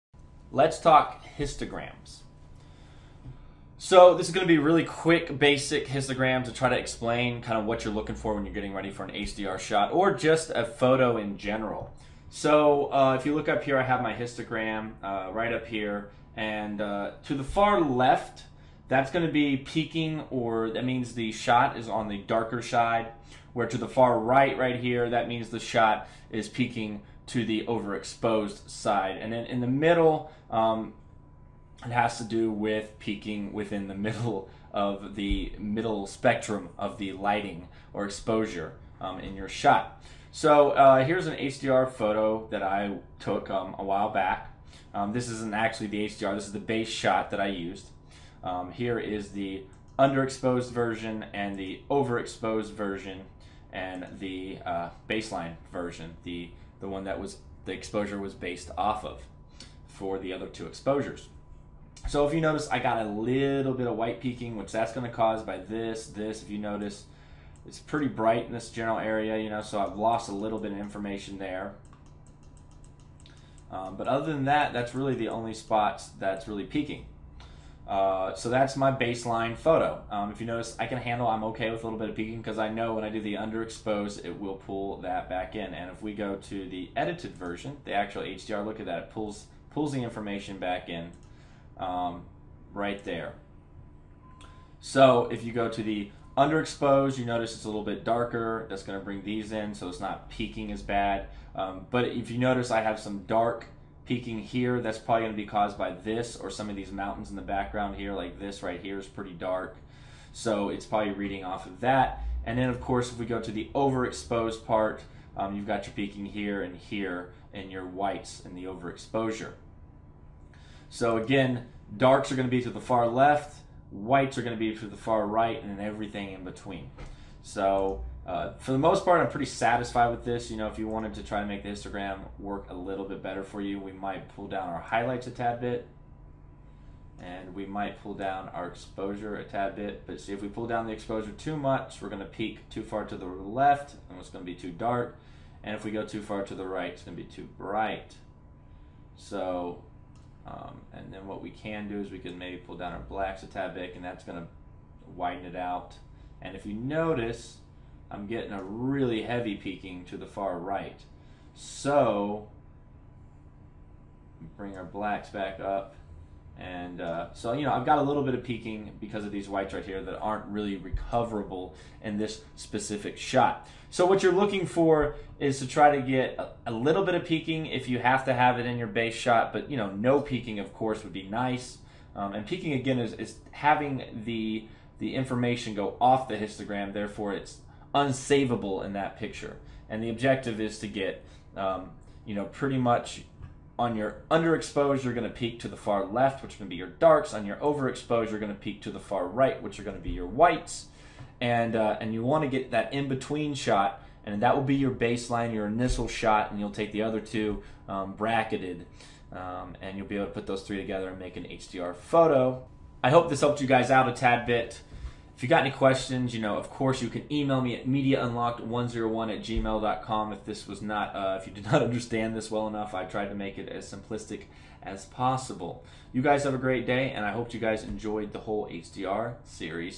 Understanding Histograms Made Easy
In this video I go over Histograms and try to explain it to you in away you can understand for people a little confused about histograms. We use Light Room to explain it.

SKYPE
Media_Unlocked